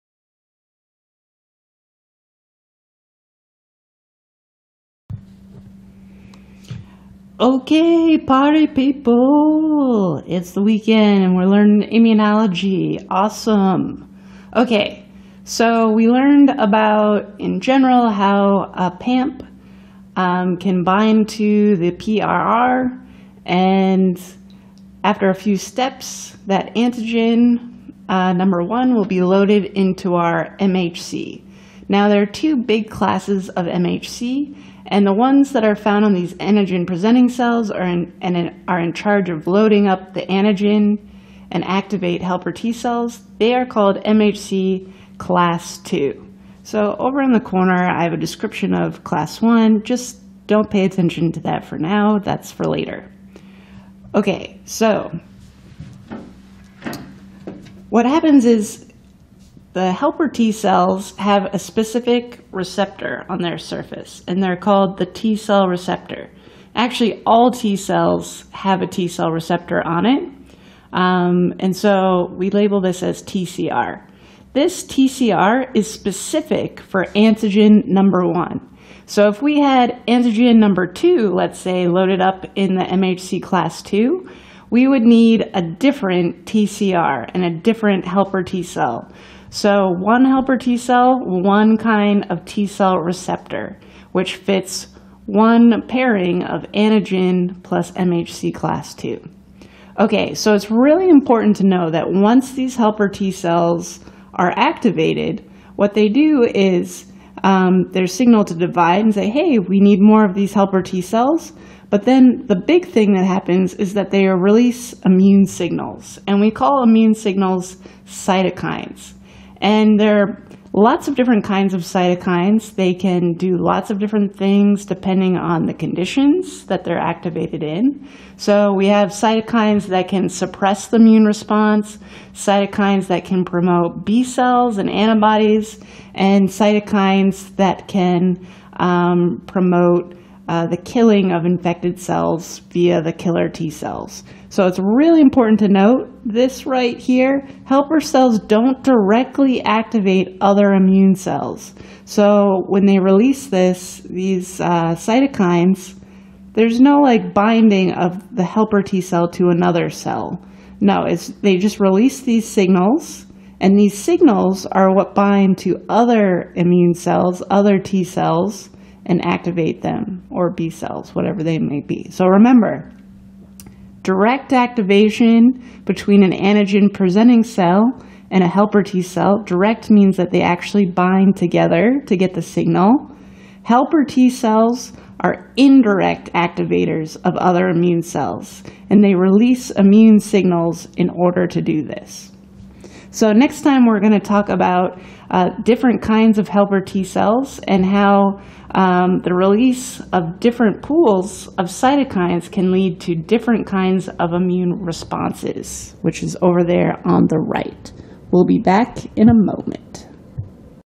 Episode 9: What do Helper T Cells Do After Activation by APC?
This video was intended for my "Communicating Science" course students who are learning how vaccines work. Before this video series, we had already covered the basics of pathogens and cell signaling.